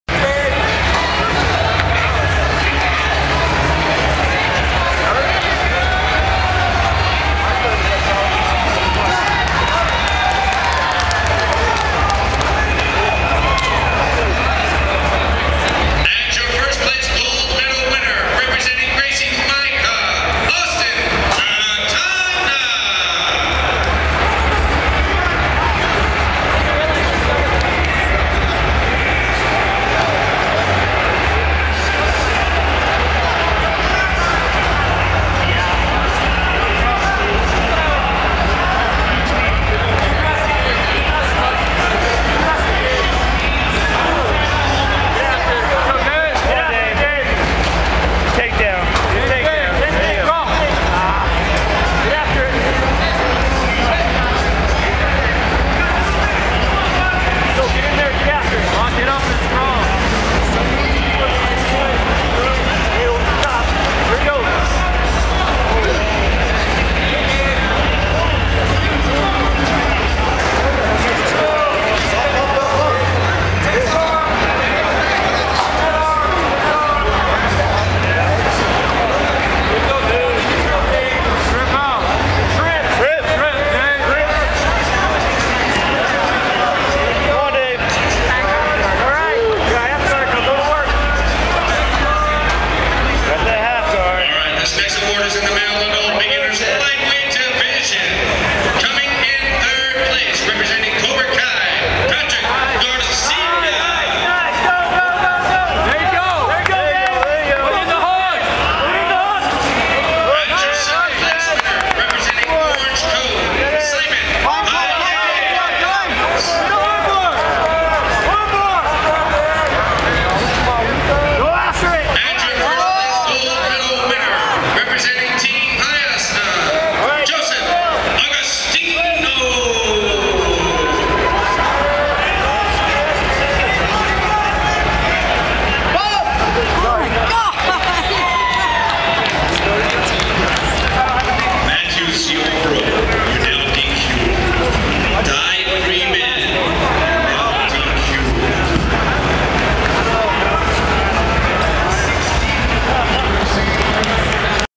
Gold medal match, Super Heavyweight Grapplers Quest 2013, Dave Ballou
This guy was very good.  It was an honor to roll with him, even if he made me look slower than cold crap on a stick.  I popped him in the back of the head twice (on accident) while attempting a triangle and armbar -- I'm lucky he didn't break my face with a punch.